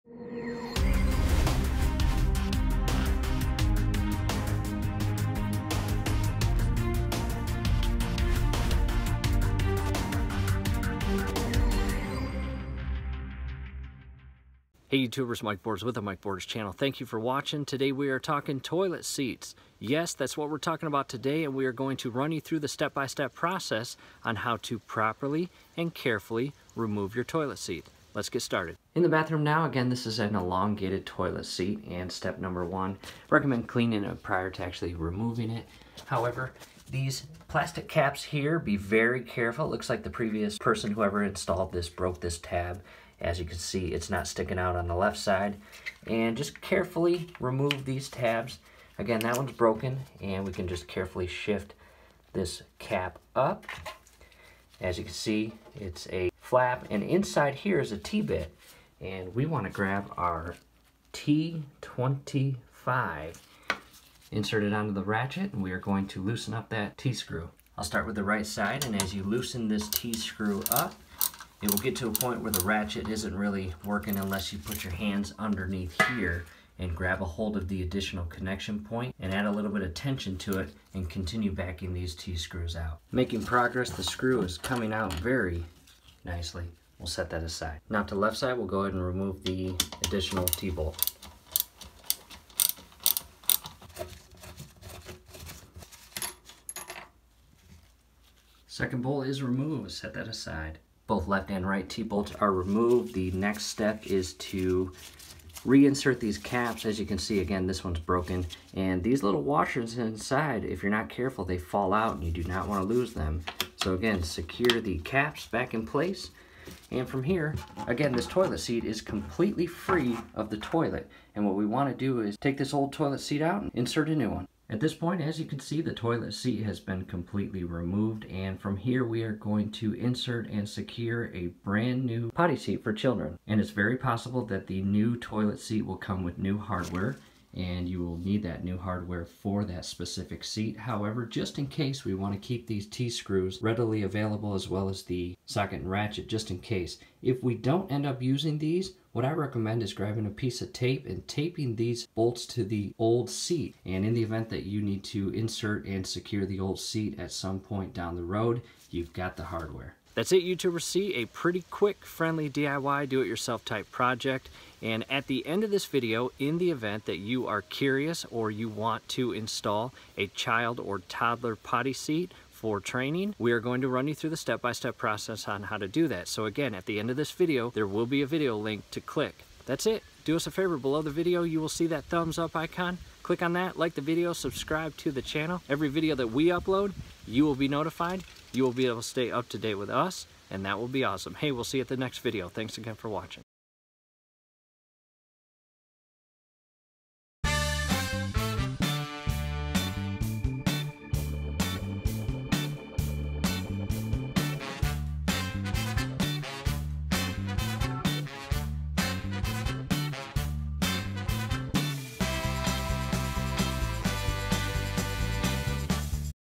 How to remove toilet seat - FAST & EASY!
Removing your toilet seat is a very friendly DIY project & can be done in just minutes!  Let's do this!